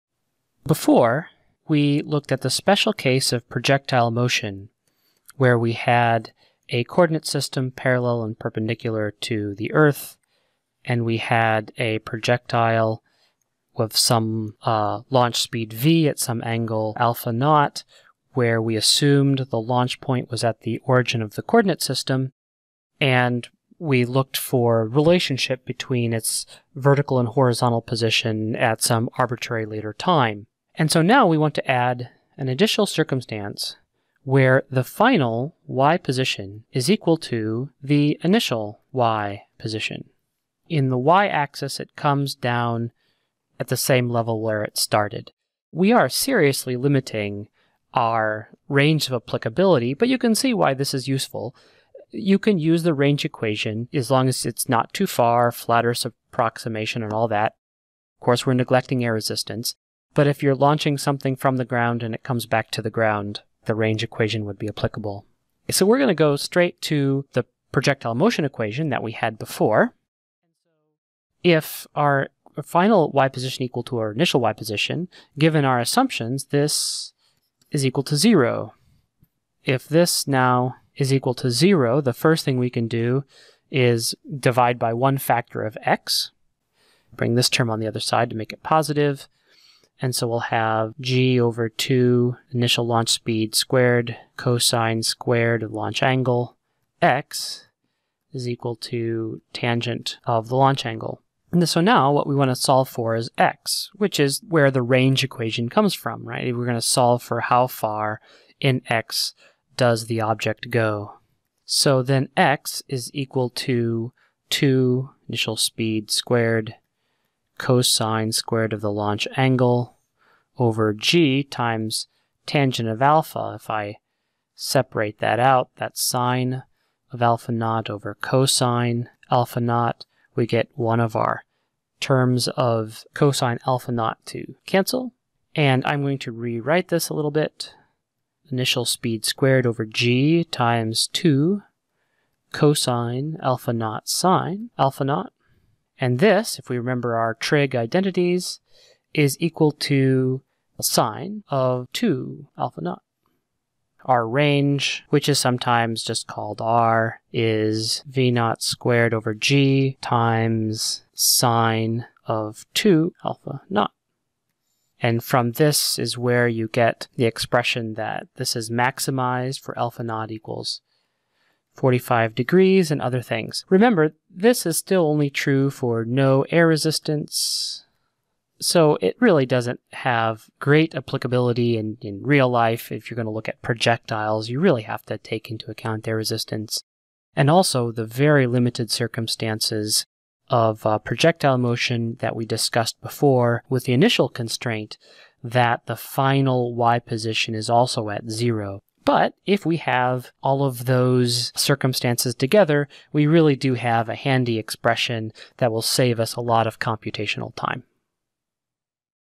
Range Equation
We derive the range equation